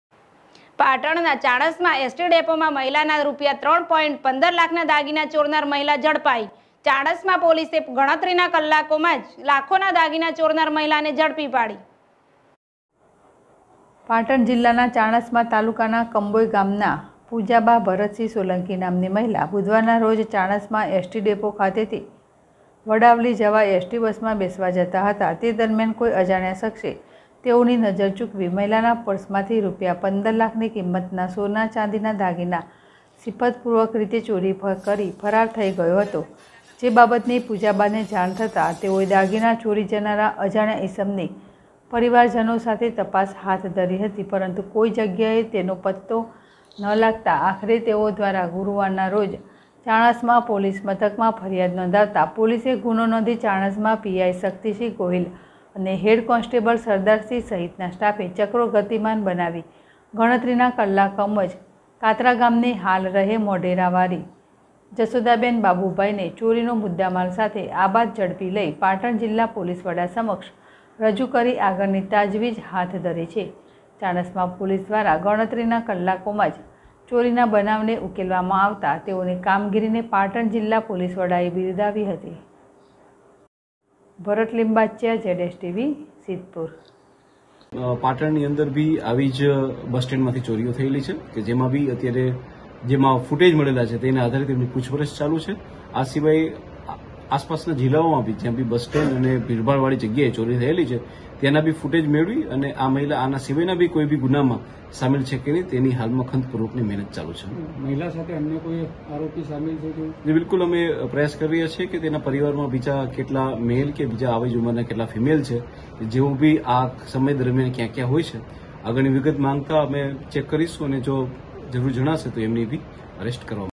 પાટણ ના ચાણસ્મા અેસ.ટી.ડેપોમાં મહિલાનાં રૂ. 3.15 લાખનાં દાગીના ચોરનાર મહિલા ઝડપાઈ
પાટણ ના ચાણસ્મા અેસ.ટી.ડેપોમાં મહિલાનાં રૂ.